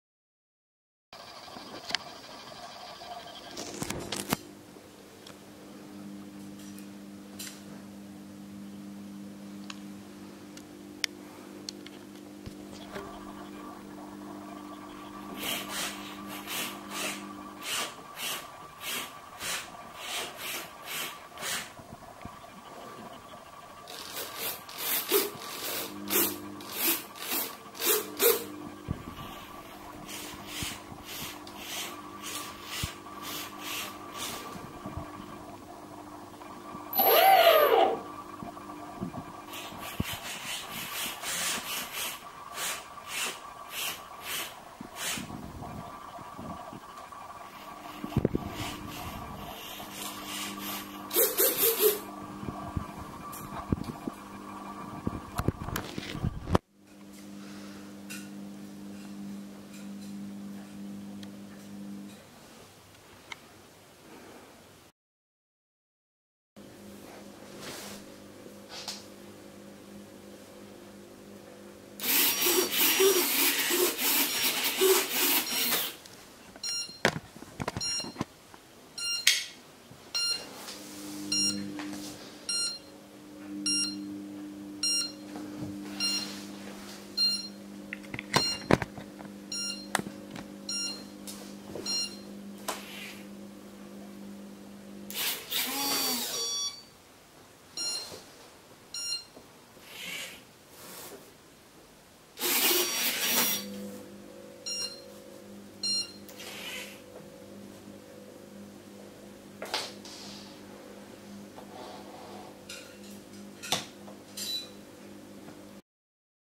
ESC Turnigy vs. Topfire
Working 45A ESC Turnigy vs. not working Topfire.